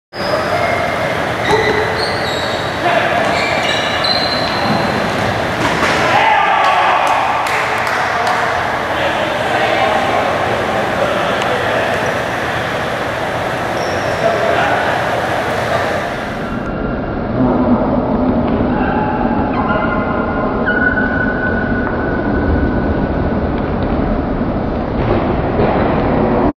Leo de Menezes Weaker Foot Goal July 08, 2013)
Leo de Menezes turns and scores the goal with his weaker foot.